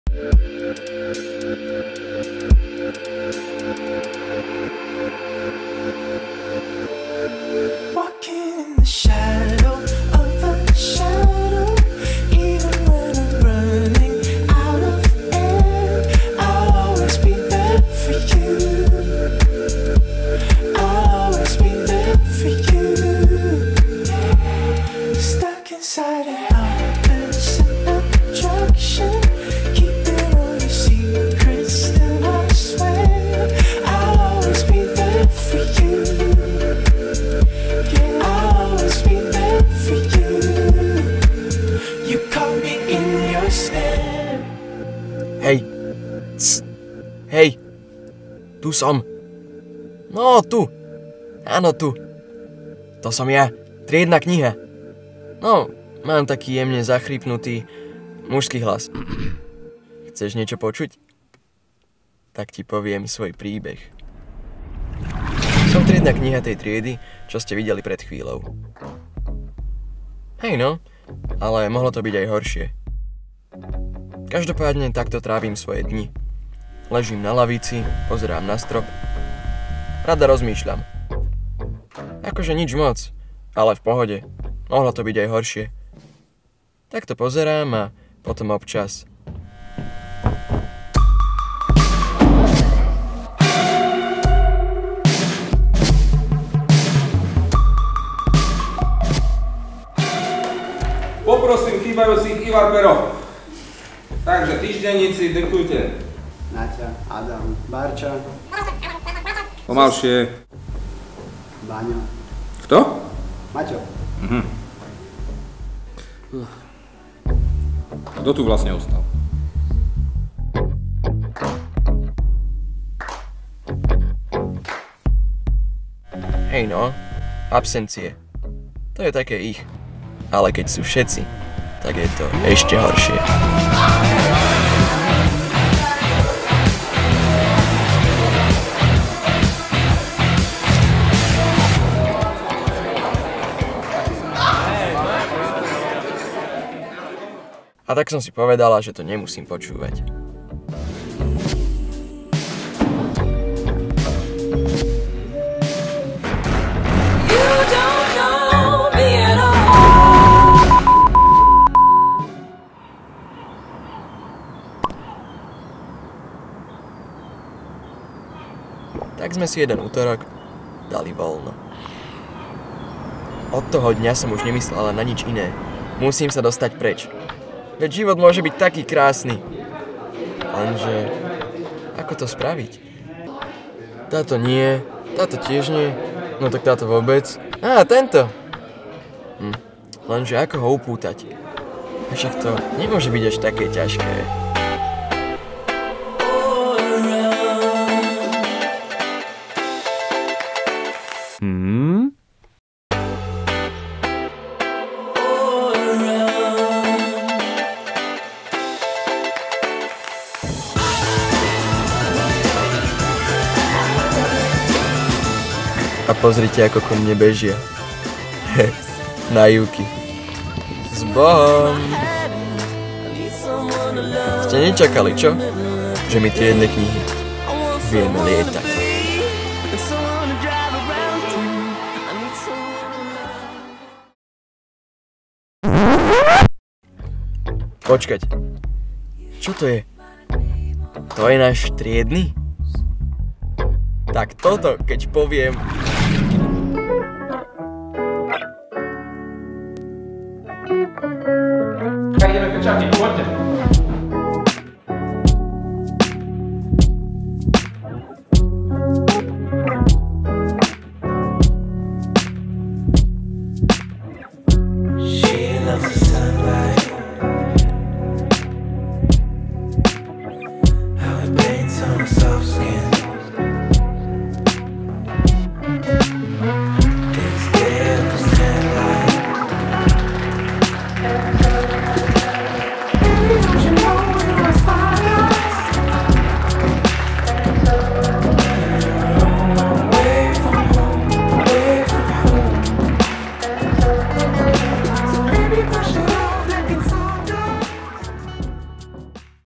Únos triednej knihy - Metodova
Krátke video únosu triednej knihy triedy Oktáva A (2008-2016) na Metodke :).

Námet a scenár:
Emil Zvarík